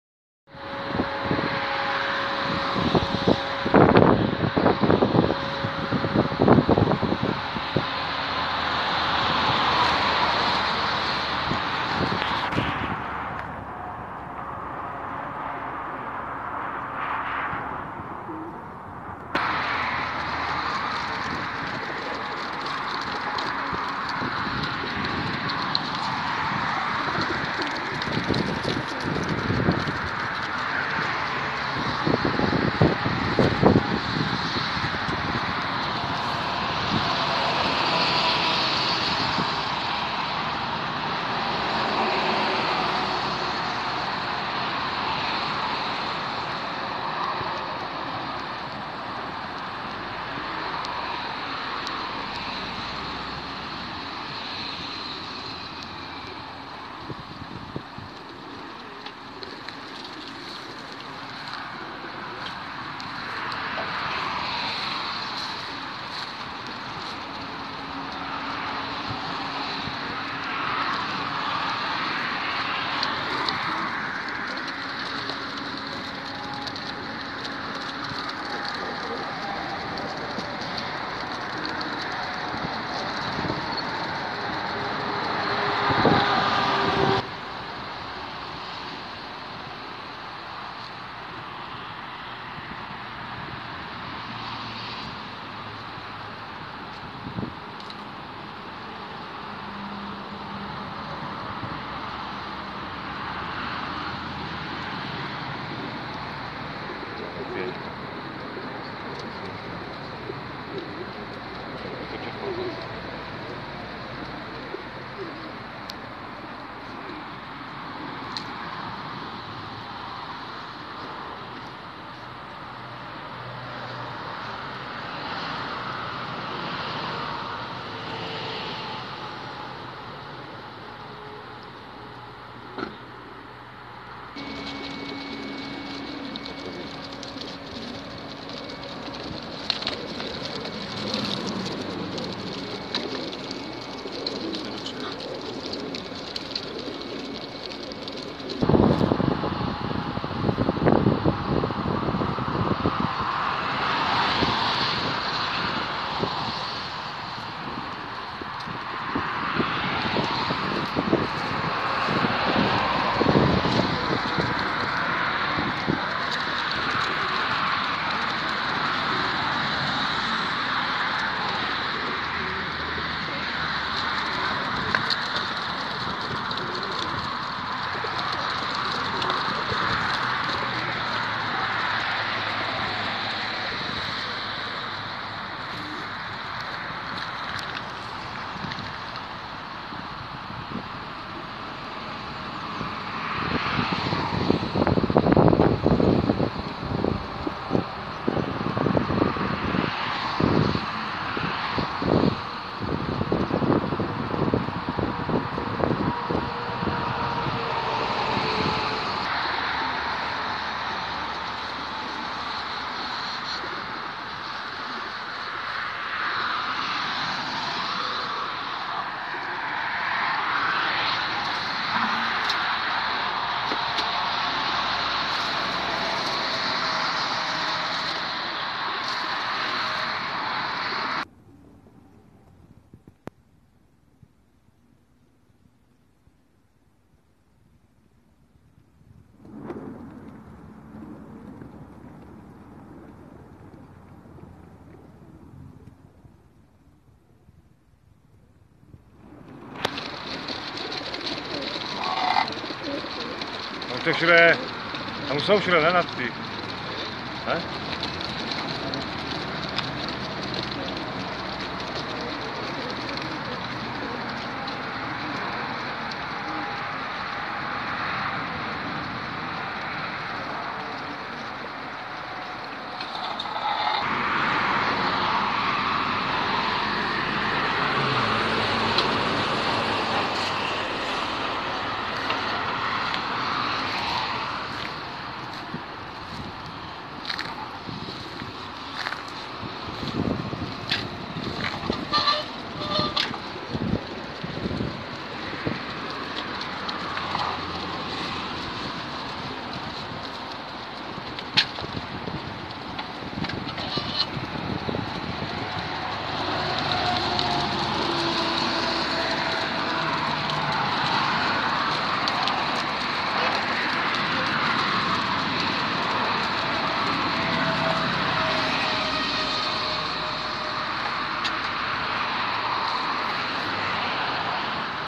ZZ Knokke 2019 3/5
ZZ Knokke 2019 3/5 - vecer napájení a krmení   200km před startem